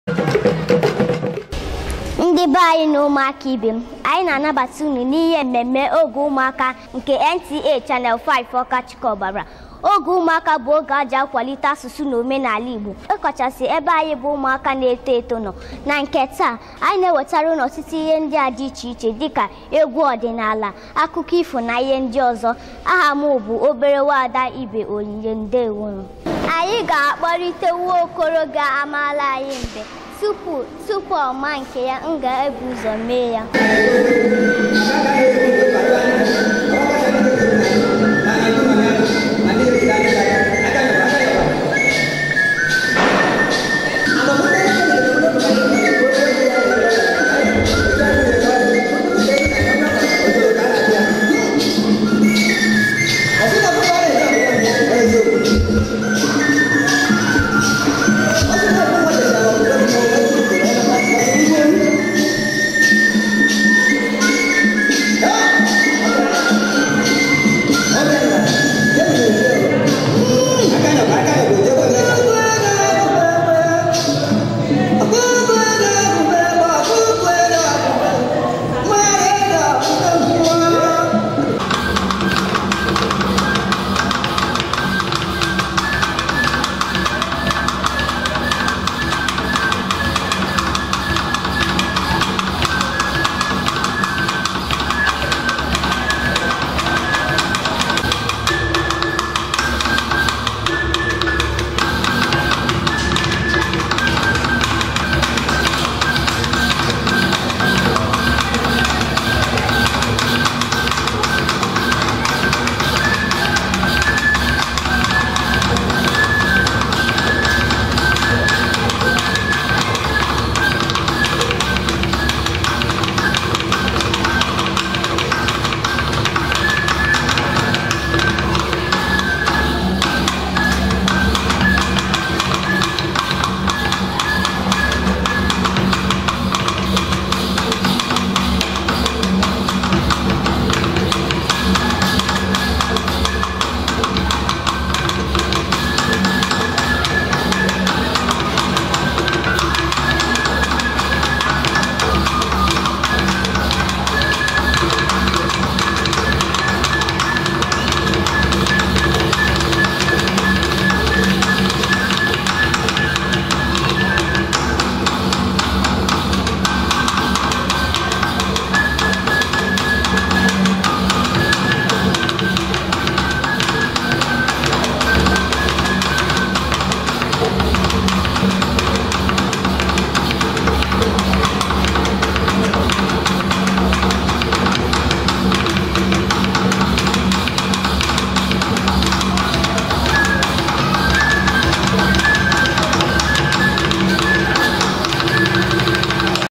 Obere nwa Ada Onyinye Ibe
We fondly call her Little Miss Favour in our Book Club. She's indeed a bookworm and we are glad she devours good books!